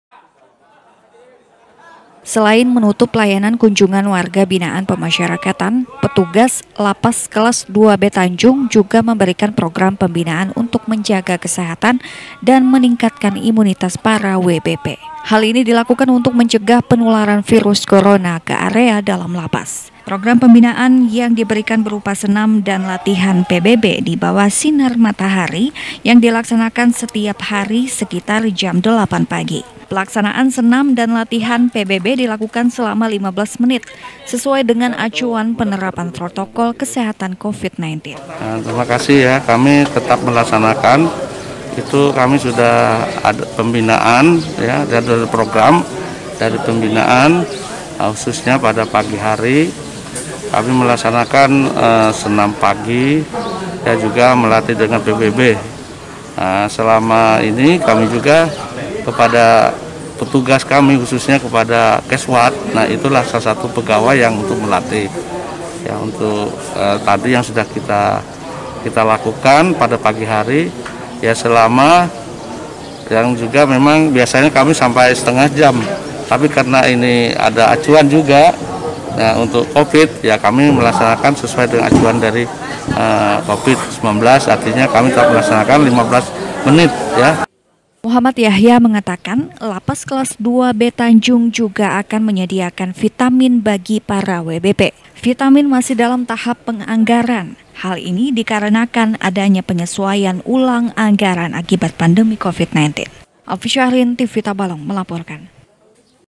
Jaga Imunitas, Warga Binaan Lapas Tanjung Rutin Olahraga
Selain Menutup Layanan Kunjungan Untuk Mencegah Penyebaran Covid Nineteen Dari Luar,Petugas Lapas Kelas Dua B Tanjung Juga Memberikan Program Pembinaan Berupa Senam Pagi Dan Latihan PBB,Untuk Menjaga Kesehatan Dan Meningkatkan Imunitas Para Warga Binaan Pemasyarakatan Lapas.